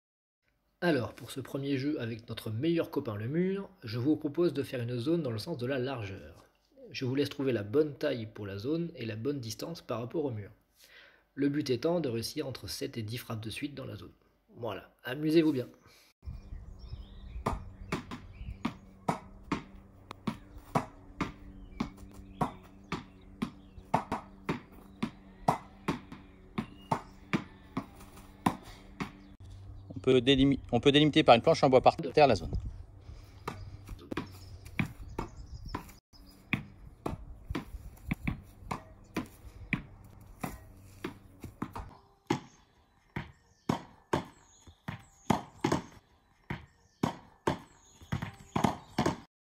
Tennis exercice à la maison, seul ou à plusieurs
voici le 2e exercice  de tennis proposé par Raphaël
une fois un peu maitrisé, on peut jouer à plusieurs, en faisant attention à ne pas se faire Mal.
Pour marquer un point, il faut avoir toucher la zone juste avant la faute de l'autre. on peut délimiter le filet par des chaises. une faute au service donne un point à l'autre.
on peut jouer avec toutes les types de balles, fonction de la place disponible et du niveau.

si vous n'avez pas de balles, elles sont commandables sur le site internet d'un magasin de sport très connu